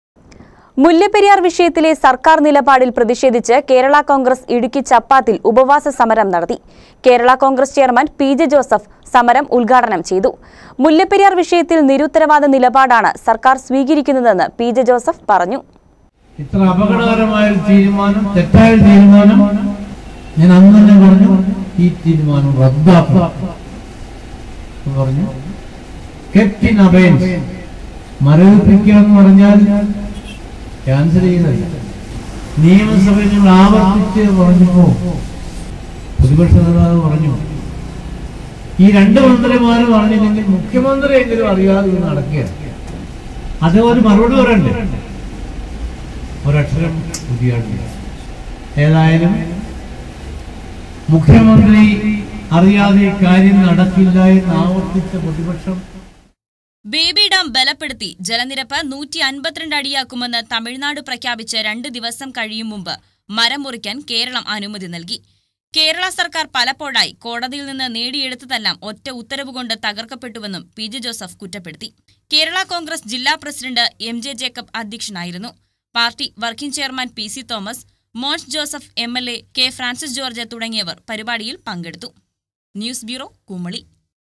ല്ലപ്പെരിയാര്‍ വിഷയത്തില്‍ നിരുത്തരവാദ നിലപാടാണ് സര്‍ക്കാര്‍ സ്വീകരിക്കുന്നതെന്ന് പി.ജെ ജോസഫ്.
മുല്ലപ്പെരിയാര്‍ വിഷയത്തിലെ സര്‍ക്കാര്‍ നിലപാടില്‍ പ്രതിഷേധിച്ച് കേരള കോണ്‍ഗ്രസ് ഇടുക്കി ചപ്പാത്തില്‍ ഉപവാസസമരം നടത്തി. കേരള കോണ്‍ഗ്രസ് ചെയര്‍മാന്‍ പി.ജെ ജോസഫ് ഉദ്ഘാടനം ചെയ്തു. മുല്ലപ്പെരിയാര്‍ വിഷയത്തില്‍ നിരുത്തരവാദ നിലപാടാണ് സര്‍ക്കാര്‍ സ്വീകരിക്കുന്നതെന്ന് പി.ജെ ജോസഫ് പറഞ്ഞു.